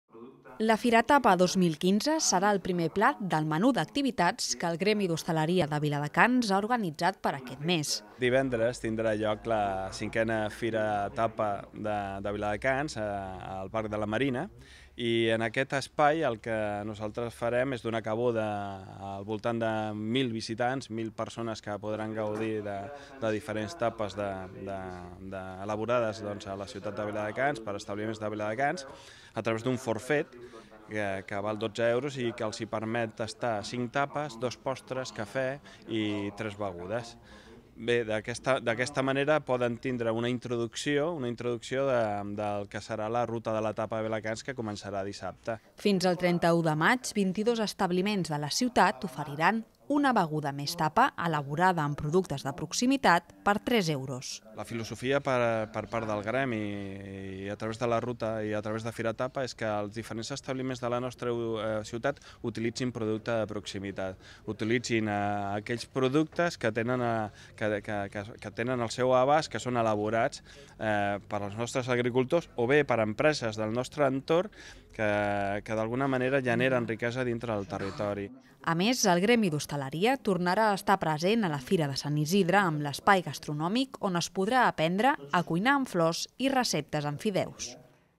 La gastronomia del Baix Llobregat es converteix en tapa aquest mes a Viladecans
La Firatapa 2015 serà el primer plat del menú d'activitats que el Gremi d'Hostaleria de Viladecans ha organitzat per aquest mes. Aquesta cita és divendres al vespre al parc de la Marina i donarà pas a la Ruta de la Tapa que es posarà en marxa l'endemà. Fins el 31 de maig 22 establiments de la ciutat oferiran una beguda més tapa elaborada amb productes de proximitat per tres euros.
A més, el Gremi d'Hostaleria tornarà a estar present a la Fira de Sant Isidre amb l'espai gastronòmic on es podrà aprendre, a cuinar amb flors i receptes amb fideus.